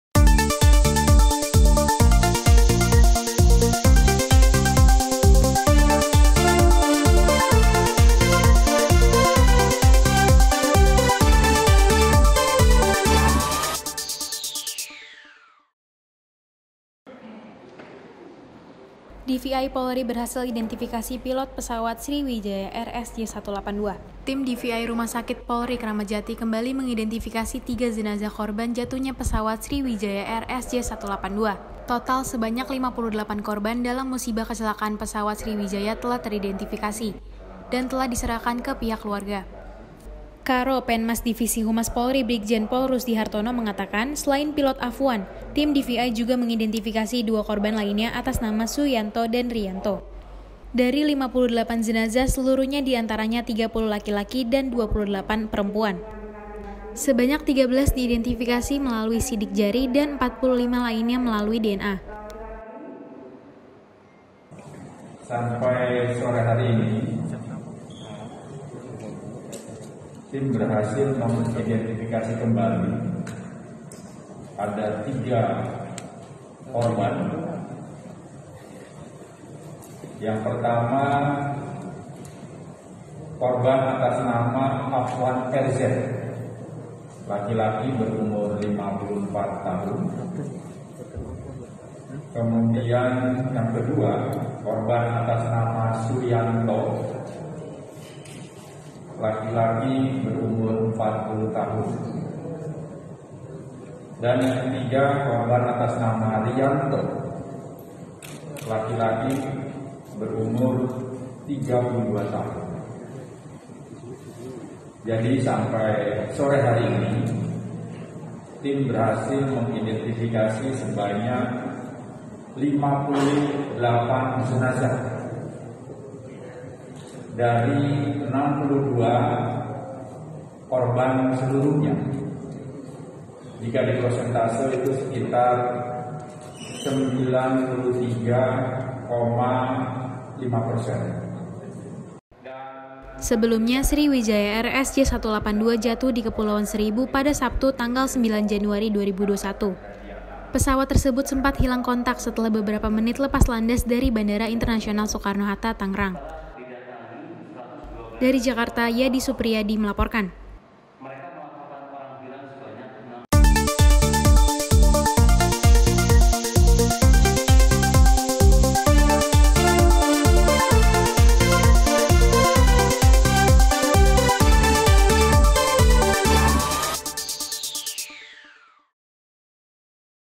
DVI POLRI BERHASIL IDENTIFIKASI PILOT PESAWAT SRIWIJAYA AIR SJ 182
Tim DVI rumah sakit Polri Kramat Jati kembali mengidentifikasi tiga jenazah korban jatuhnya pesawat Sriwjaya SJ-182. Total sebanyak 58 korban, dalam musibah kecelakaan pesawat Sriwijaya telah teridentifikasi, dan telah diserahkan ke pihak keluarga.

Karo Penmas Divisi Humas Polri Brigjen Pol Rusdi Hartono mengatakan selain Pilot Afwan, tim DVI juga mengidentifikasi dua korban lainnya atas nama Suyanto dan Riyanto.

Dari 58 jenazah seluruhnya di antaranya 30 laki-laki dan 28 perempuan. Sebanyak 13 diidentifikasi melalui sidik jari dan 45 melalui DNA. Sebelumnya Sriwijaya Air SJ 182 jatuh di Kepulauan Seribu pada Sabtu tanggal 09 Januari 2021, Pesawat disebut sempat hilang kontak setelah beberapa menit lepas landas dari Bandara Internasional Soekarno-Hatta, Tangerang.

Tim liputan :Supriyadi
Melaporkan.